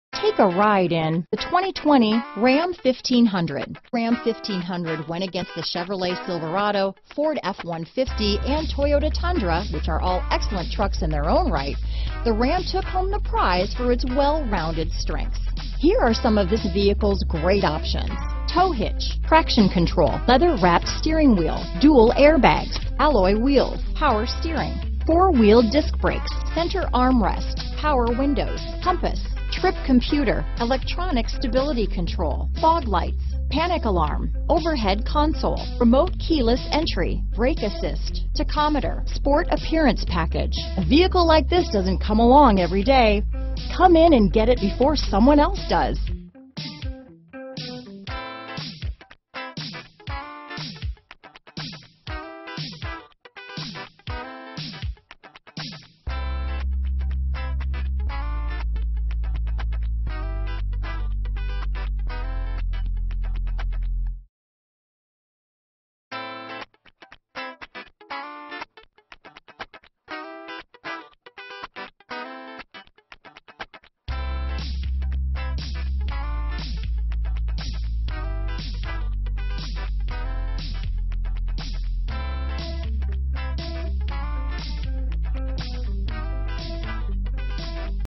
2020 Ram 1500 New Smyrna Beach, Port Orange, Daytona Beach, Deltona, Sanford, FL N107693
Crystal Metallic New 2020 Ram 1500 available in New Smyrna Beach, Florida at New Smyrna Chrysler. Servicing the Port Orange, Daytona Beach, Deltona, Sanford, FL area.
2020 Ram 1500 Big Horn/Lone Star - Stock#: N107693 - VIN#: 1C6RREFG2LN107693